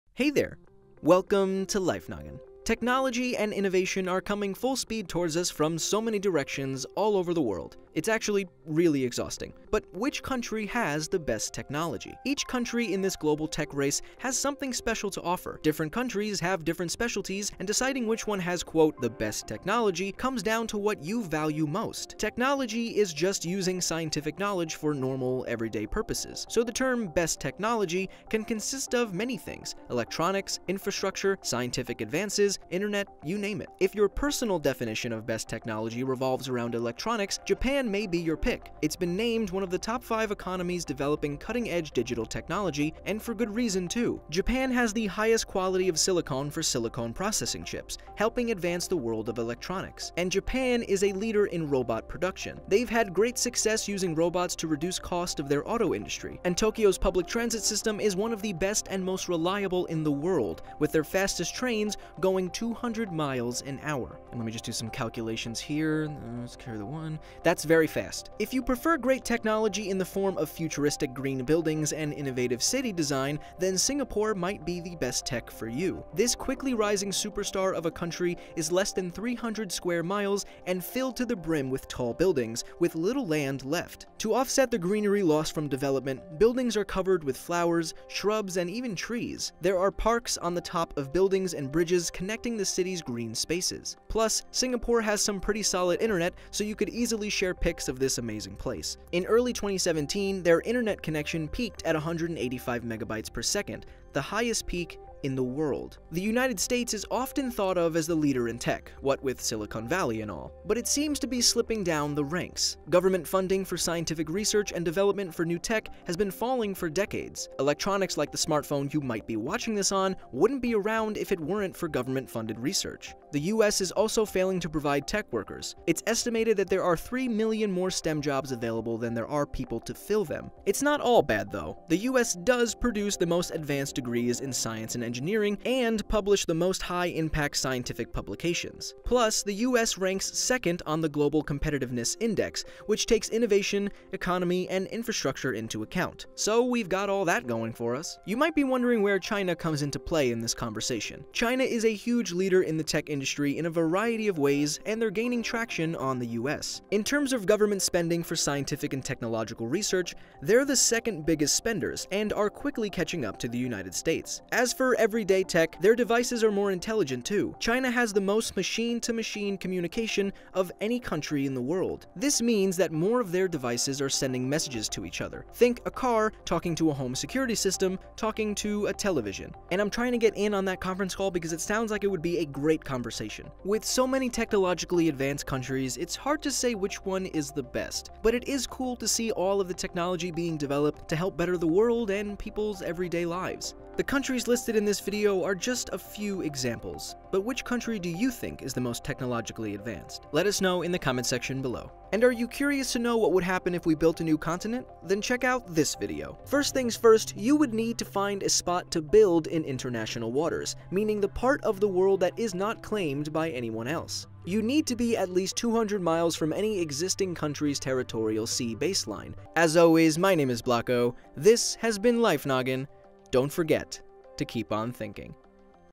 Which Country Has The Best Technology?
Technology is advancing at a crazy rate. But which country is leading the way?